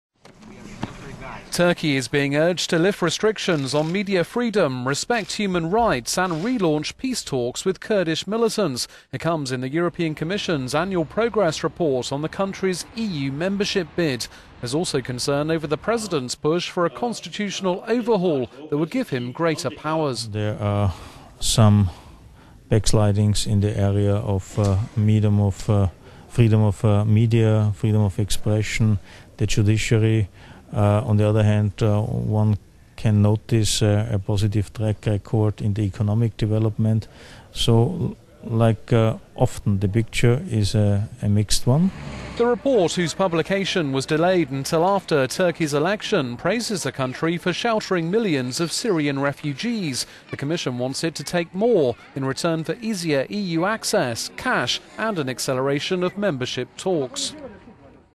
Turkey: EU report highlights 'backslidings' in media freedom and judiciary
Turkey is being urged to lift restrictions on media freedom, respect human rights and relaunch peace talks with Kurdish militants. It comes in the European Commission's annual progress report on the country's EU membership bid.

There is also concern over President Tayyip Erdogan's push for a constitutional overhaul that would give him greater powers.

But the EU executive's criticisms are cloaked in diplomatic language, reflecting the political sensitivity of dealing with a large, strategicall…